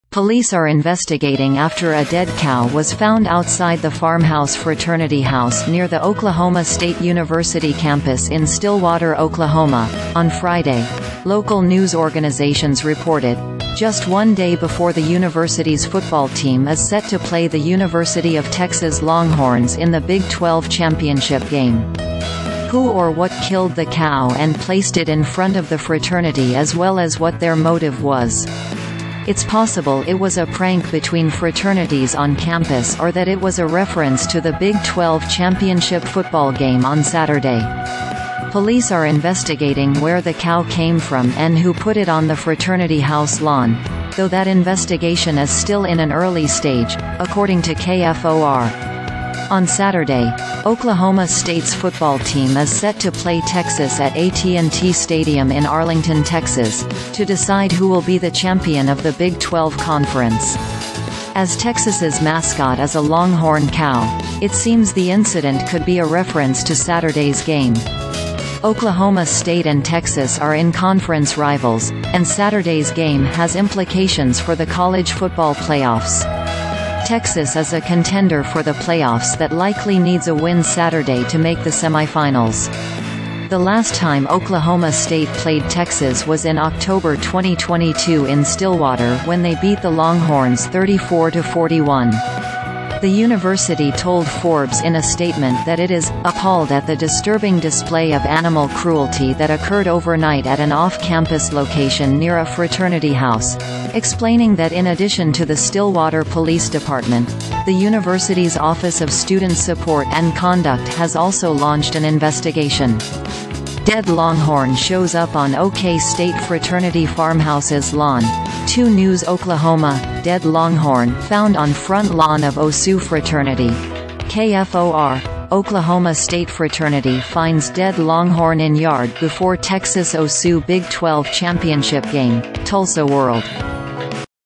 Dead Cow Found In Front Of Oklahoma State Fraternity House Ahead Of Big
Police are investigating after a dead cow was found outside the FarmHouse fraternity house near the Oklahoma State University campus in Stillwater, Oklahoma, on Friday, local news organizations reported, just one day before the university’s football team is set to play the University of Texas Longhorns in the Big 12 championship game. Who or what killed the cow and placed it in front of the fraternity as well as what their motive was. It’s possible it was a prank between fraternities on campus or that it was a reference to the Big 12 Championship football game on Saturday. Police are investigating where the cow came from and who put it on the fraternity house lawn, though that investigation is still in an early stage, according to KFOR. On Saturday, Oklahoma State’s football team is set to play Texas at AT&T Stadium in Arlington, Texas, to decide who will be the champion of the Big 12 Conference. As Texas’ mascot is a longhorn cow, it seems the incident could be a reference to Saturday’s game. Oklahoma State and Texas are in-conference rivals, and Saturday’s game has implications for the College Football Playoffs. Texas is a contender for the playoffs that likely needs a win Saturday to make the semifinals. The last time Oklahoma State played Texas was in October 2022 in Stillwater when they beat the Longhorns 34-41. The university told Forbes in a statement that it is “appalled at the disturbing display of animal cruelty that occurred overnight at an off-campus location near a fraternity house," explaining that in addition to the Stillwater Police Department, the university’s office of student support and conduct has also launched an investigation. Dead longhorn shows up on OK State fraternity FarmHouse's lawn (2 News Oklahoma)Dead longhorn found on front lawn of OSU fraternity (KFOR)Oklahoma State fraternity finds dead longhorn in yard before Texas-OSU Big 12 championship game (Tulsa World)